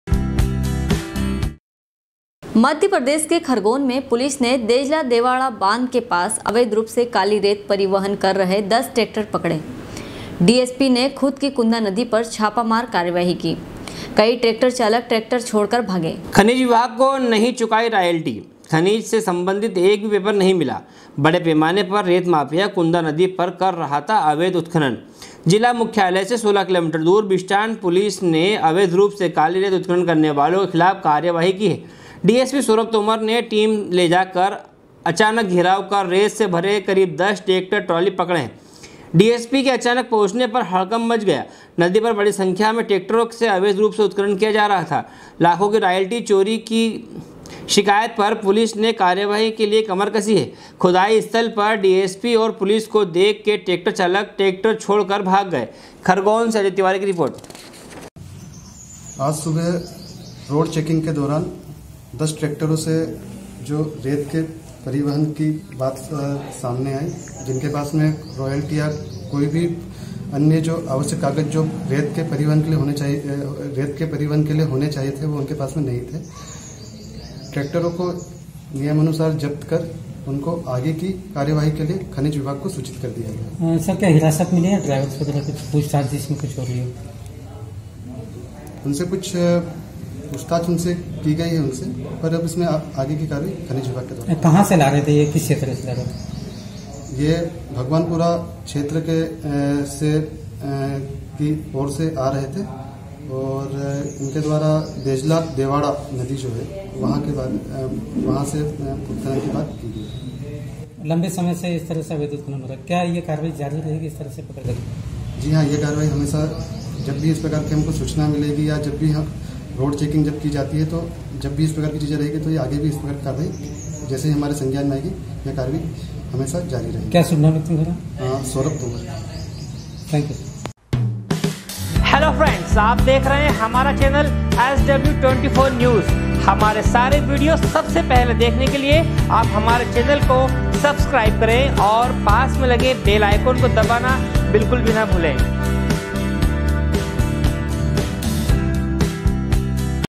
SW 24 News Madhyapradesh
मध्यप्रदेश के खरगोन में पुलिस ने देजला- देवाड़ा बांध के पास अवैध रूप काली रेत परिवहन कर रहे 10 ट्रैक्टर पकड़े। डीएसपी ने खुद की कुंदा नदी पर छापामार कार्यवाही। कई ट्रैक्टर चालक ट्रेक्टर छोड़कर भागे।